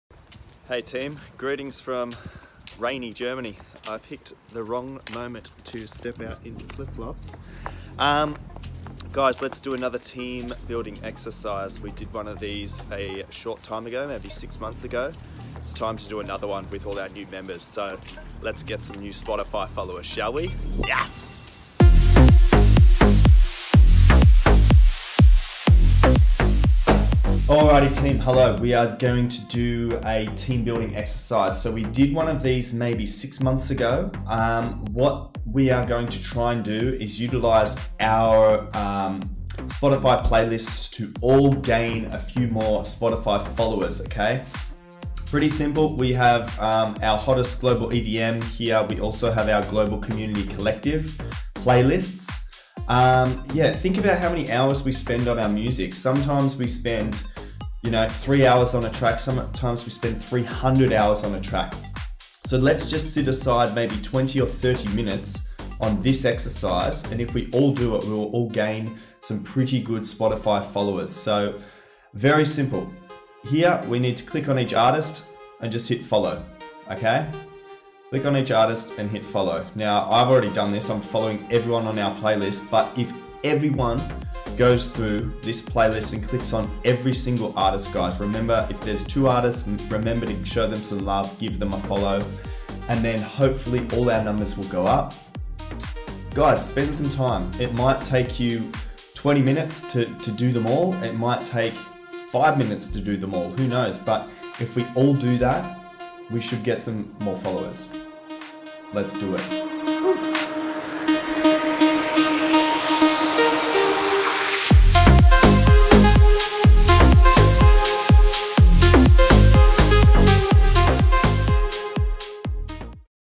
The Power of Discord and Spotify
Let's use our discord community to gain more Spotify followers...